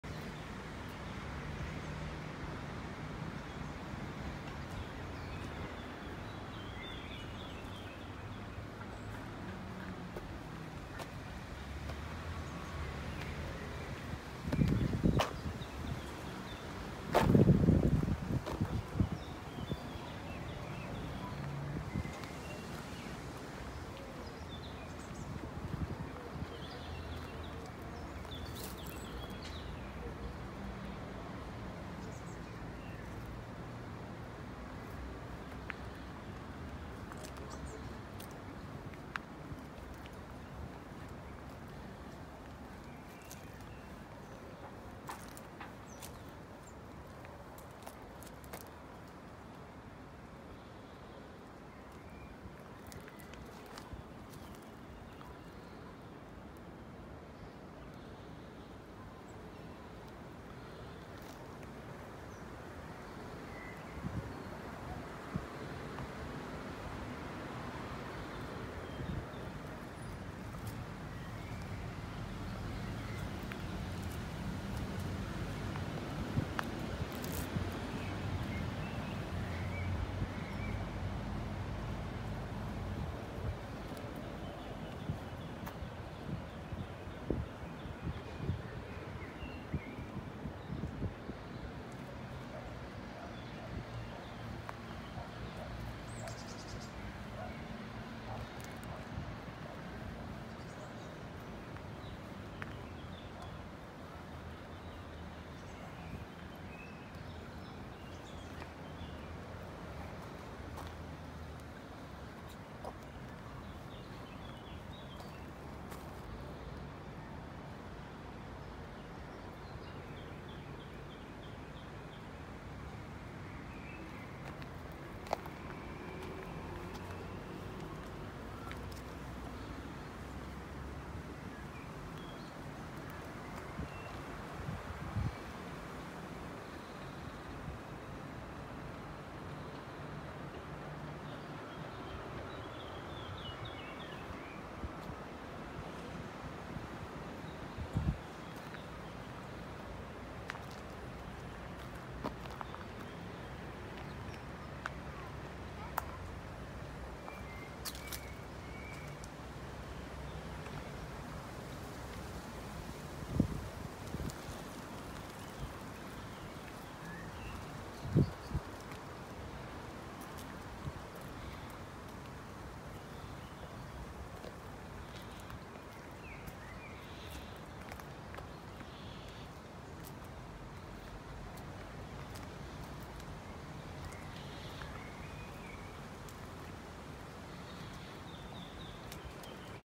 Giardini Lamboglia nel degrado a Genova Marassi
Genova - Sporcizia e mancanza di controlli all’interno dei giardini Lamboglia di Marassi